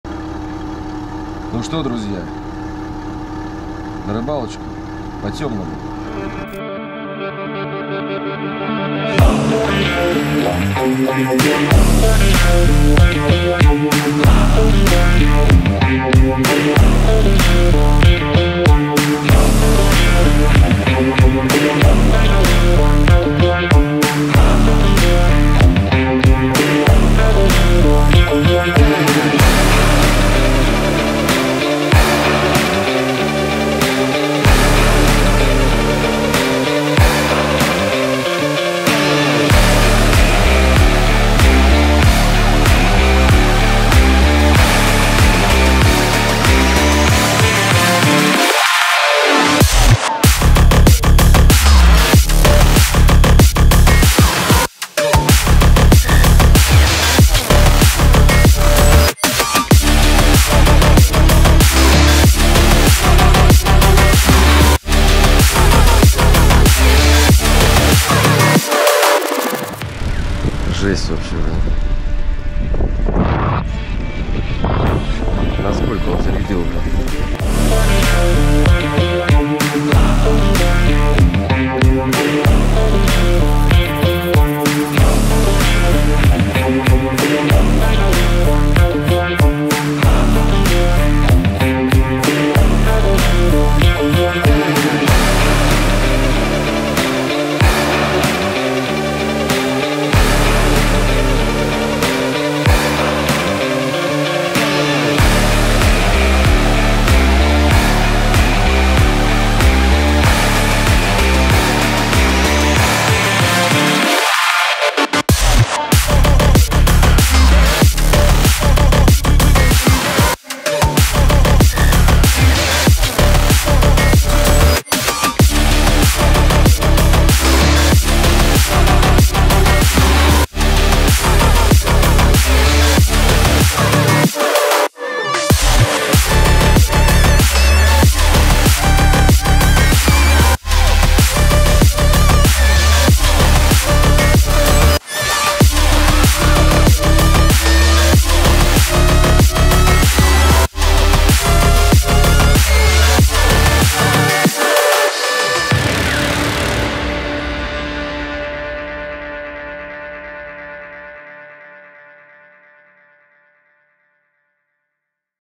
ОСЕННЯЯ ПОЕЗДКА на реку Ахтуба, рыбалка в Ноябре 2022 года. трейлер новинки
- группа в ВК отчёты с рыбалок, где и на что поймана рыба